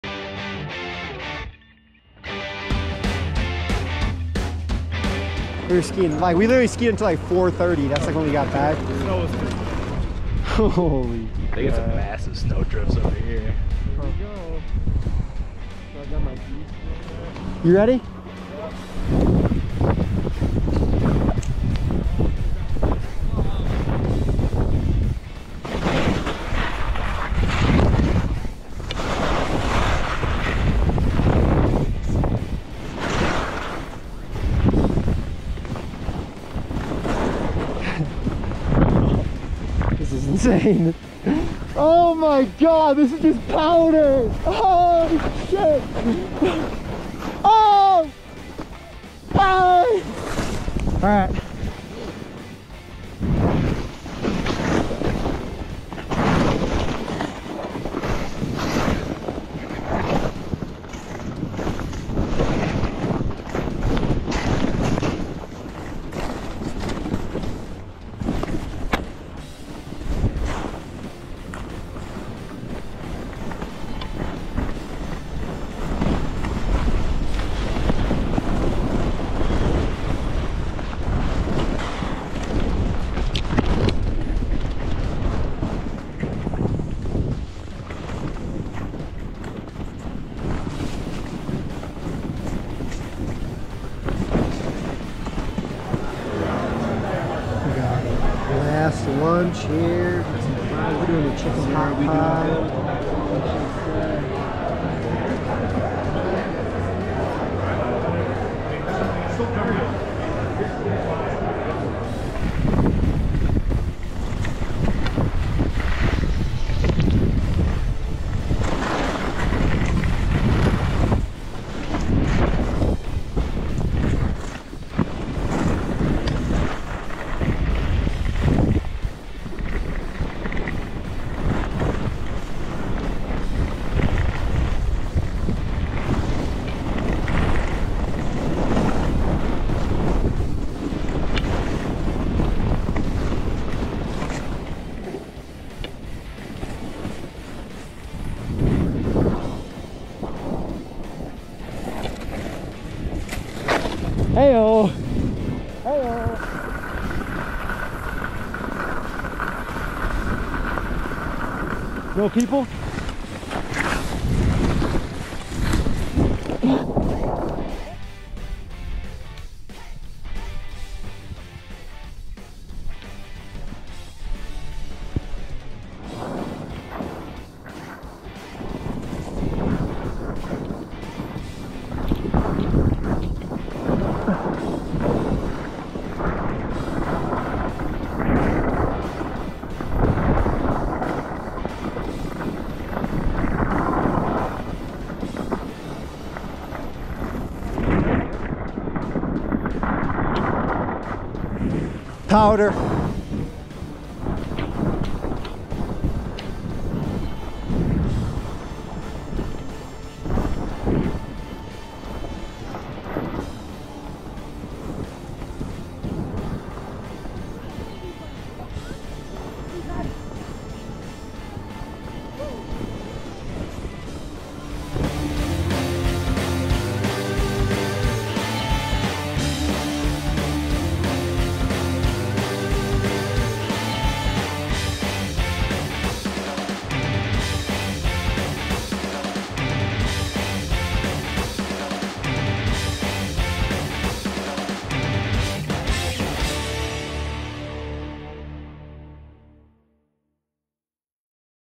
Insane Skiing at 12,000 ft! Winter Park, CO - Day 4
This was the last day at winter park resort in winter park, CO. We went all the way to the peak of the mountain via the panoramic lift and had an awesome day skiing at over 12,000 ft. I’m going to keep posting more content from the trip so stay tuned!